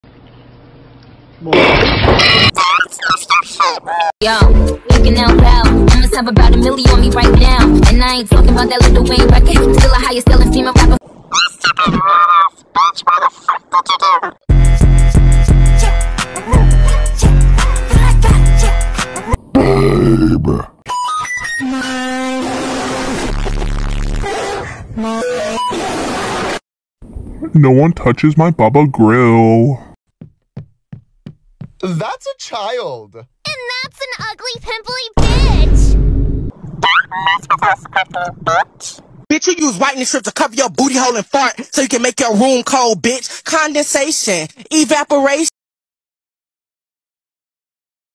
Reacting to gacha cringe pt 4 (the worst video I found was on tiktok not YouTube)
no warning this already has dumb stuff